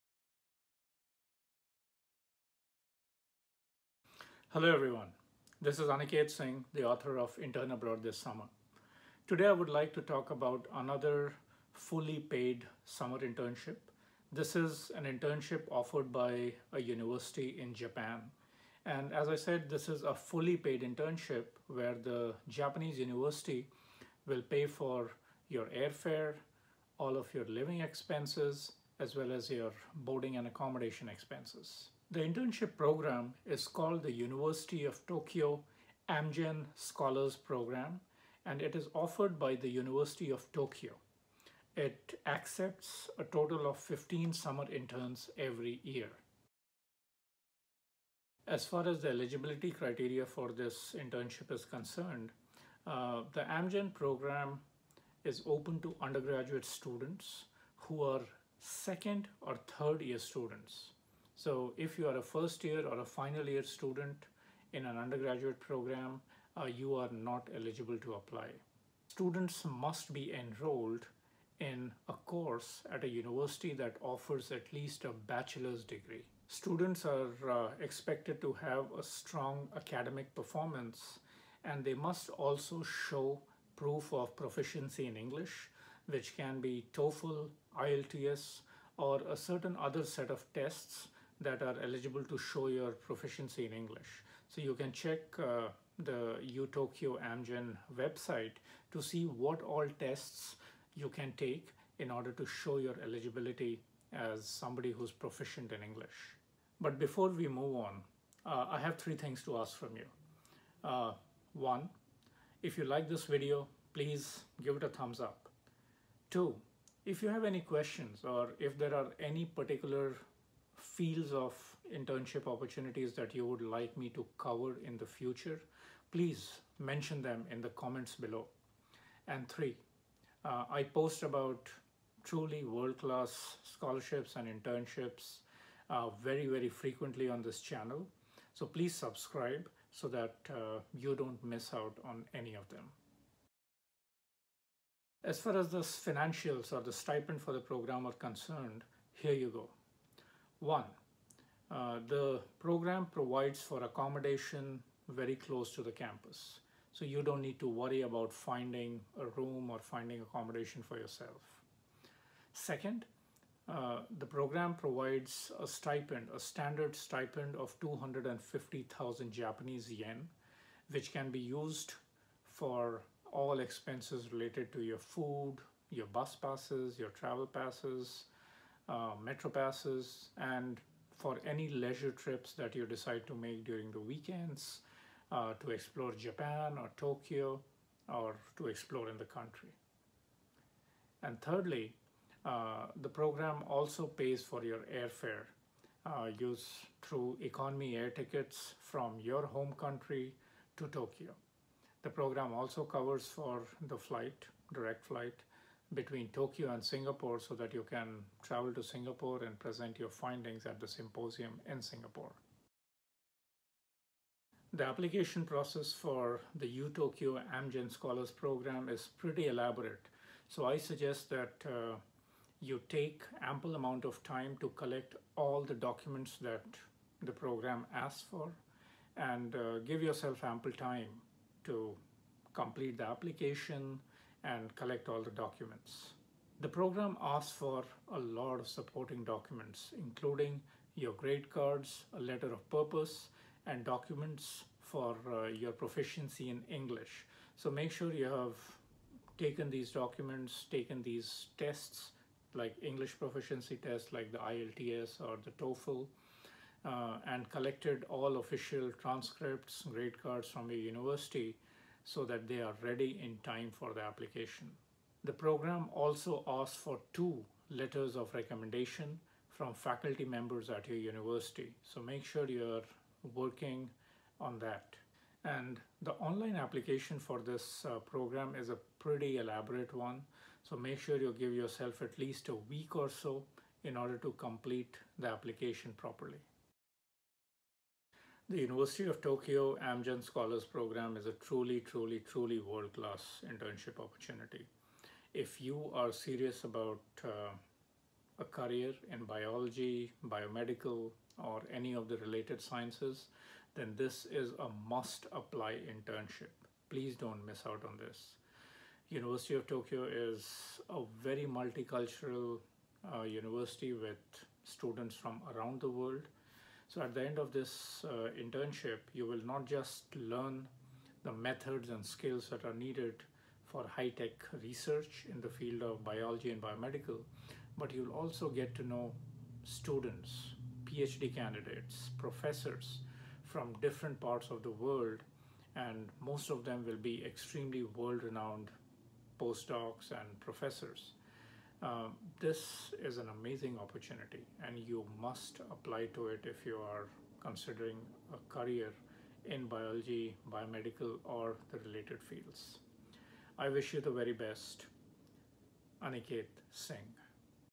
Summer Internship : Part 2 : Tokyo Amgen Scholars - Fully Paid Summer Internship
University of Tokyo Amgen Scholars Program - Fully Paid Summer Internship (Aniket Singh - Intern Abroad This Summer)

Learn About the Eligibility/Application/Funding.

To learn about hundreds of such life changing internships:
The Complete Book of International Internships
The Complete Book of Internships in India